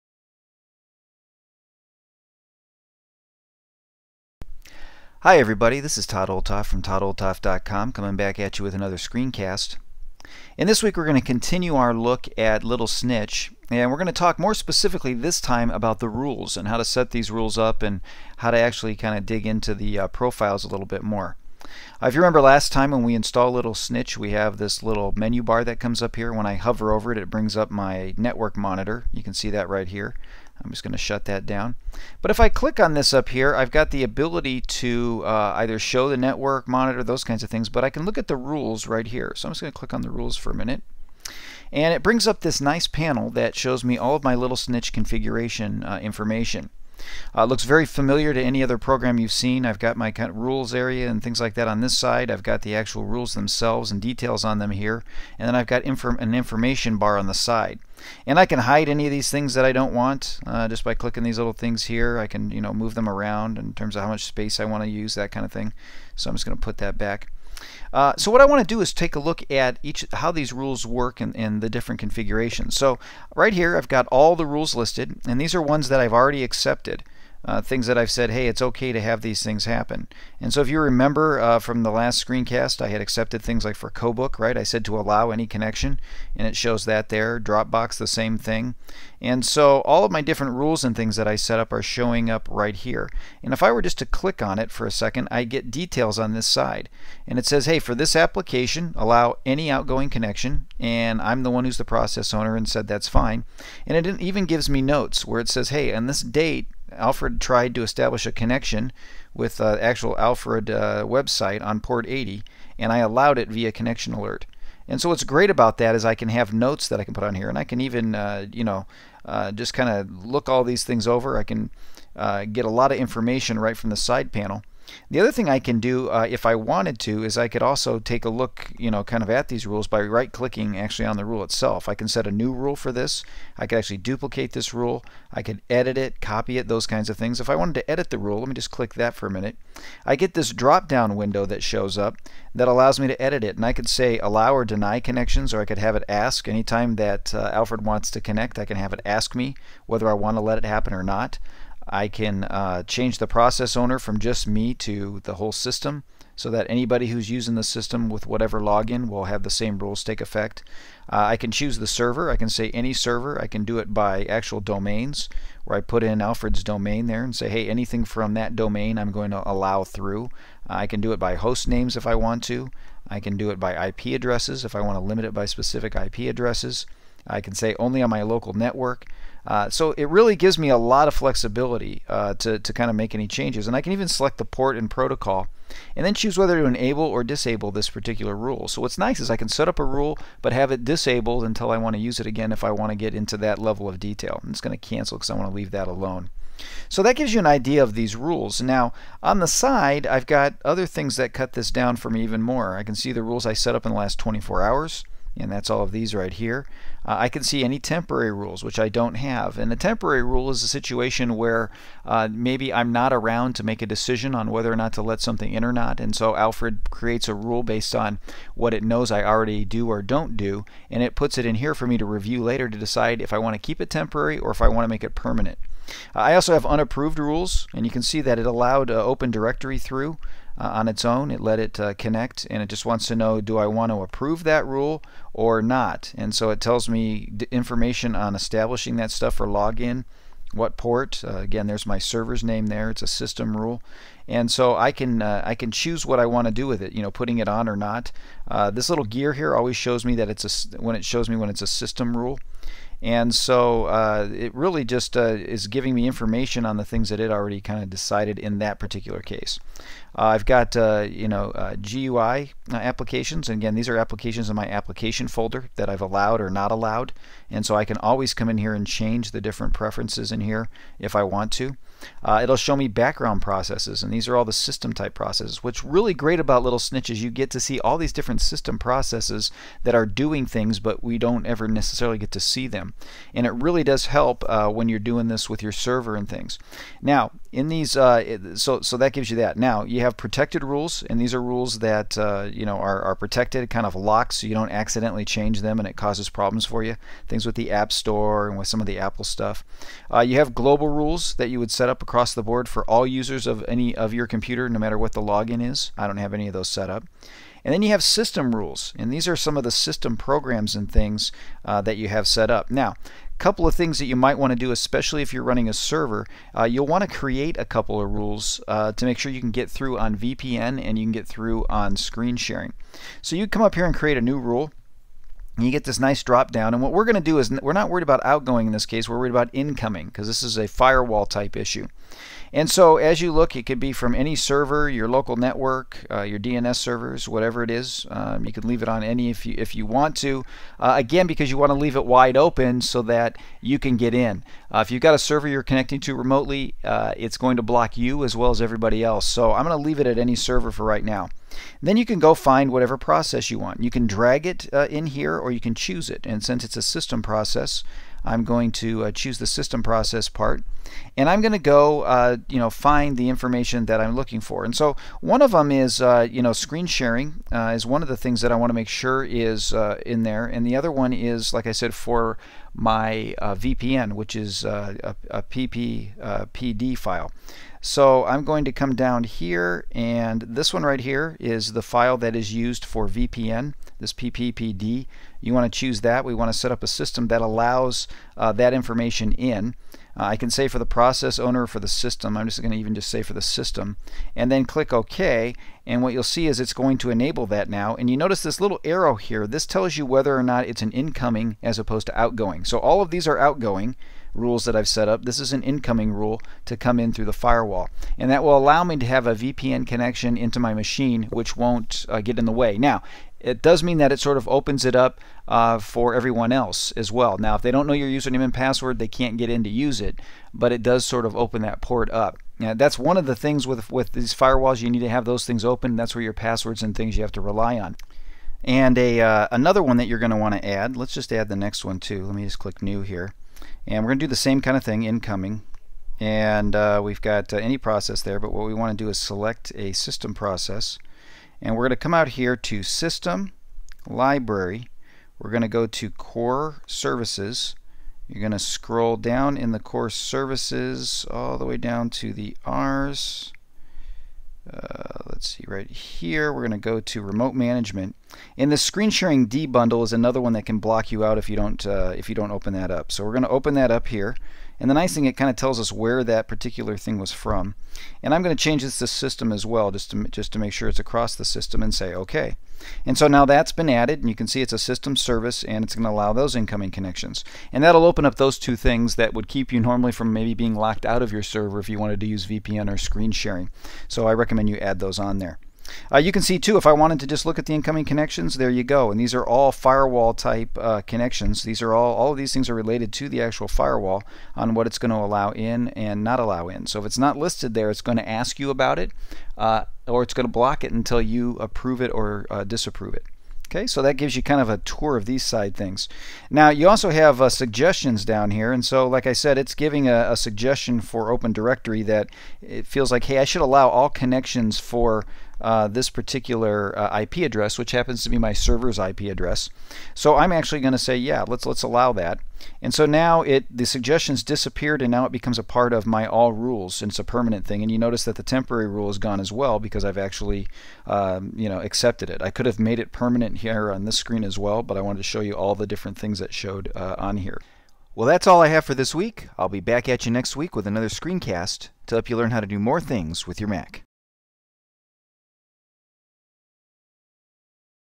Mountain Lion Server Part 46: Little Snitch Part 2-Rules
In this screencast tutorial, I continue my look at Little Snitch by taking a closer look at how rules are managed and set up in the software. I cover the various categories of rules from incoming to outgoing rules as well as rules to put in place to make sure you can remotely log into your server through VPN or screen sharing (VNC).

If you find these screencasts helpful please like, favorite, and share them so others can find them as well:).